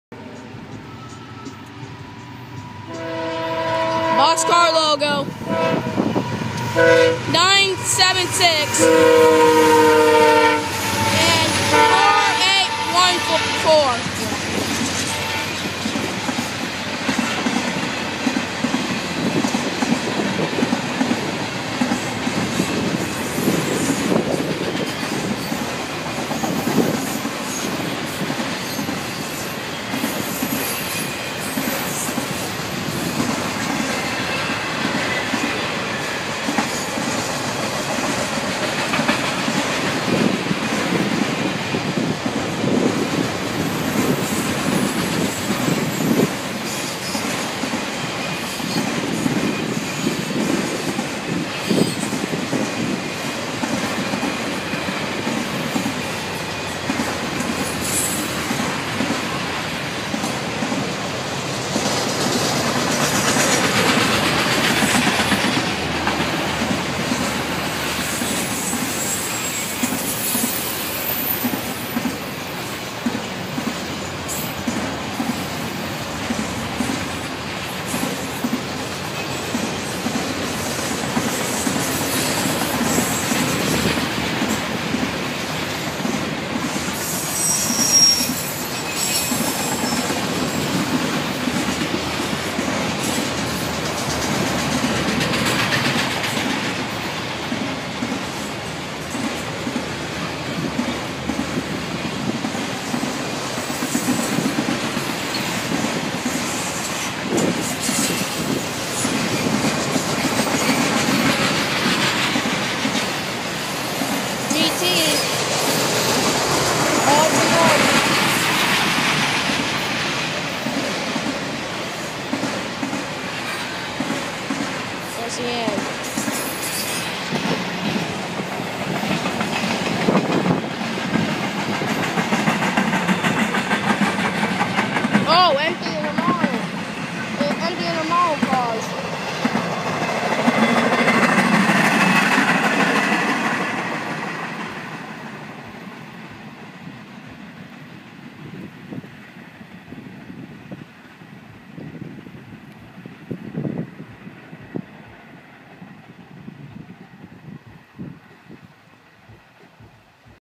Trackmasterkid5 Railfanning Box Car Logo CSX leading CSX 4814 at Selmaville RD.
A great catch of CSX that day my 3rd Box Car Logo CSX catch incuding CSX 4814, which was in a train derailment a few year ago (That engine got stuck in a lake)